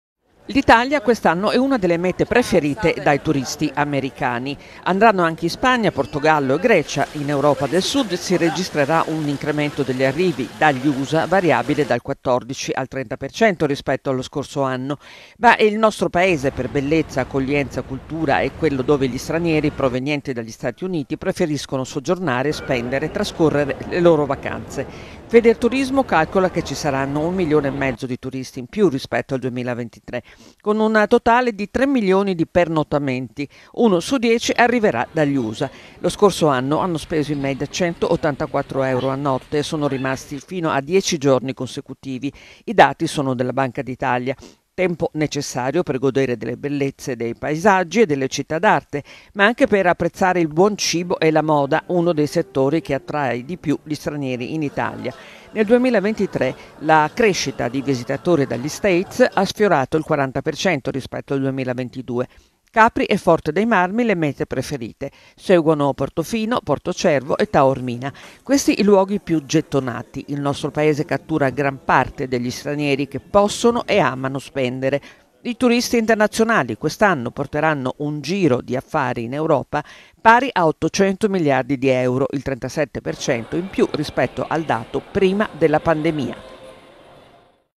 Turismo, boom di presenze dagli Usa
L'Italia è una delle mete preferite dai turisti americani, che sceglieranno anche Spagna, Portogallo e Grecia. Il Sud Europa registrerà così un incremento degli arrivi dagli Usa variabile dal 14 al 30% rispetto al 2023. Sul podio il Belpaese per bellezza, accoglienza, cultura. Federturismo calcola che ci sarà un milione e mezzo di turisti in più rispetto allo scorso anno, con un totale di 3 milioni di pernottamenti. E uno su 10 arriverà proprio dagli Stati Uniti.